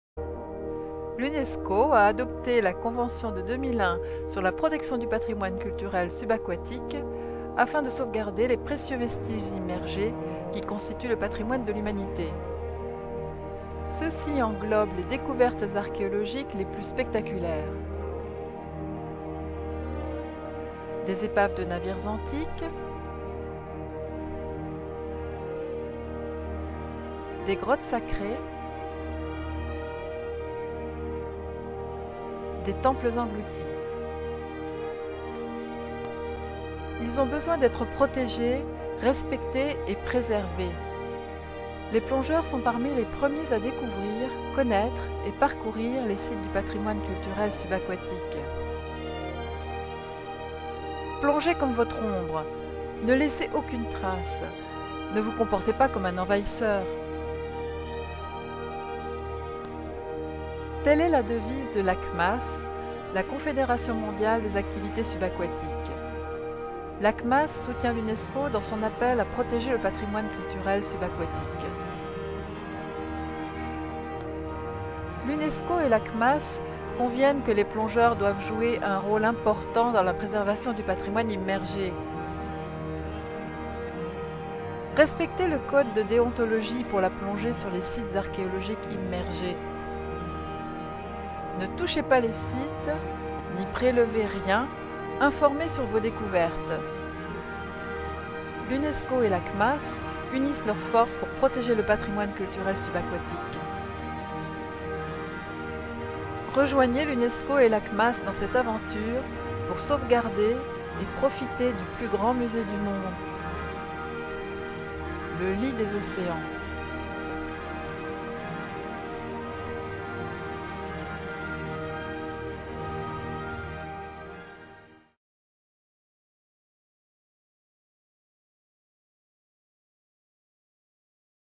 L'UNESCO et la CMAS collaborent pour protéger le patrimoine culturel subaquatique
L'UNESCO et la Confédération Mondiale des Activités Subaquatiques (CMAS) unissent leurs forces pour la sauvegarde du patrimoine culturel subaquatique. Ils conviennent que les plongeurs doivent jouer un rôle important dans la préservation du patrimoine subaquatique.